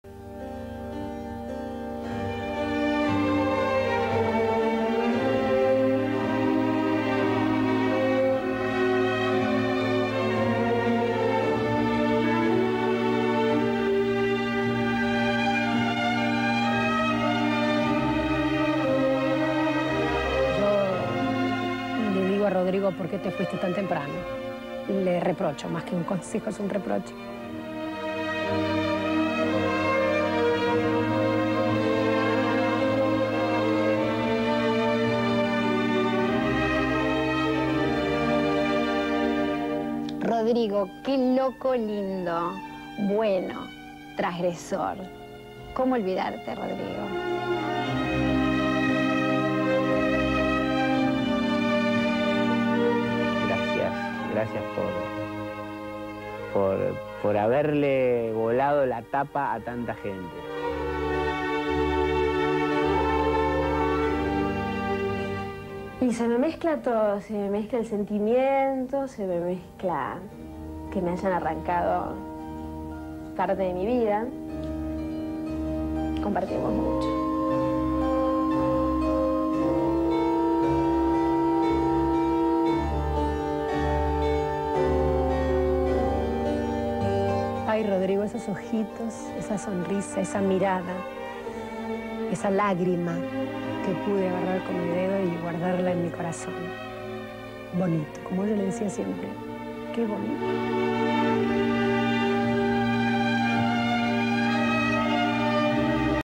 Carmen Barbieri, Mónica De Alzaga, Camilo García y Marixa Balli hablan sobre Rodrigo (2000)
Especial homenaje a Rodrigo del canal América en el año 2000